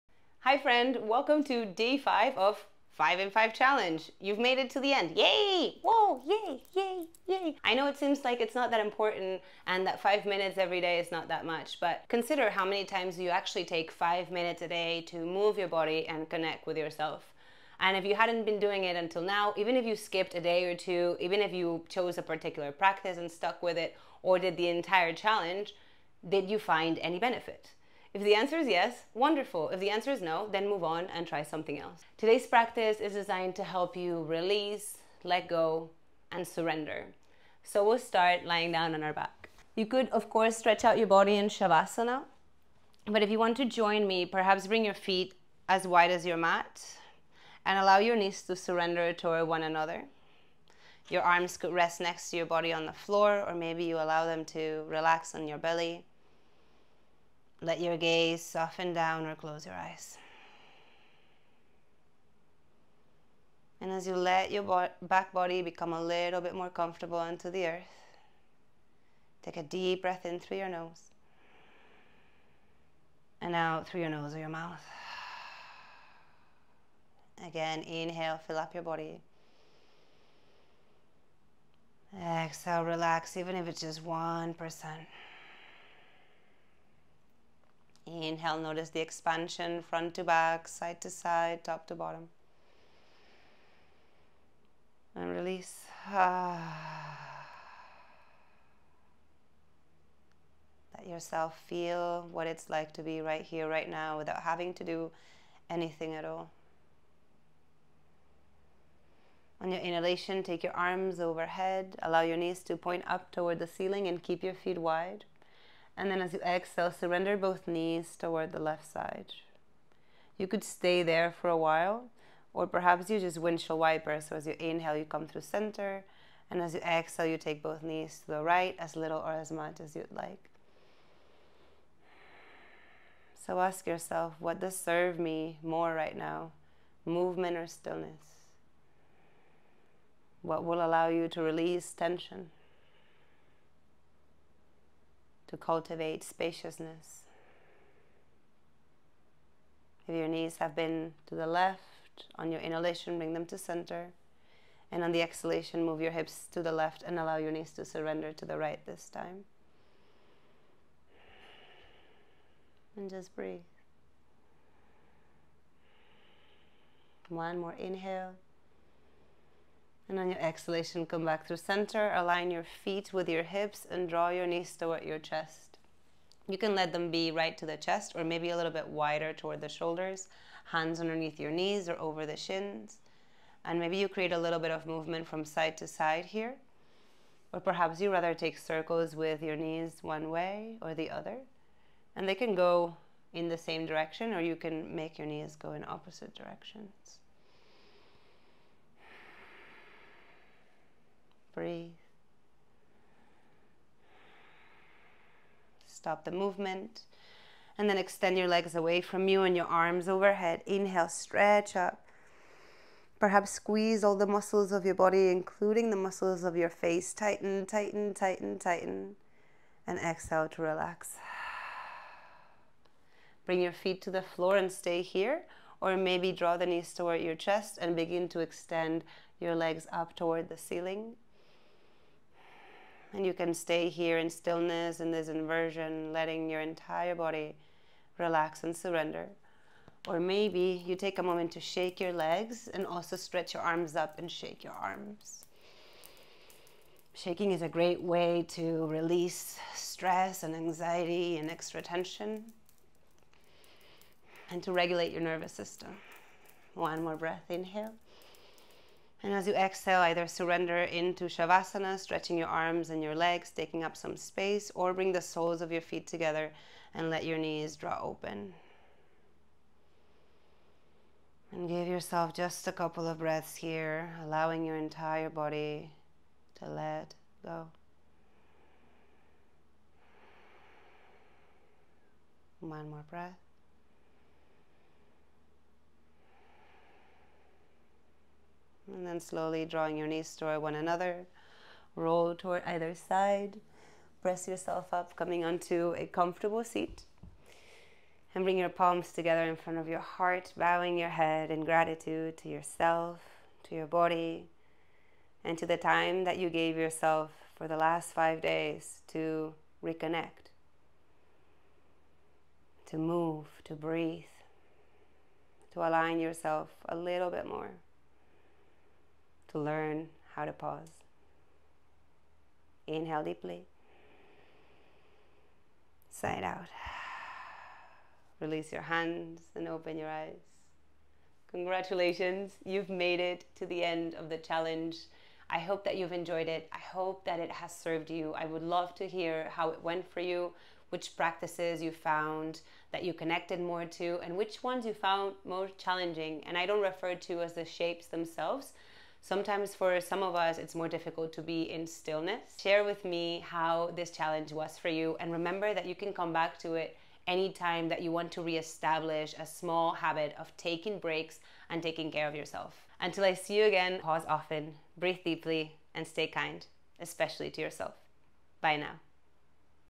5 in 5 Yoga Challenge Day 5 - Release | 5 Min Yoga for Stress Relief
Congratulations on making it to the last day of the challenge!

Whether you've shown up every day or this has taken you a bit longer, you should be super proud. Today we celebrate by releasing stress. Let yourself soften into this short yet nurturing practice.

Thank you for having joined me for 5 days of movement, awareness, and learning to pause.

Timestamps:
0:00 - Intro
0:38 - Centering
1:50 - Floor Sequence
4:30 - Relaxation
4:52 - Closing

get more resources to support you and join the community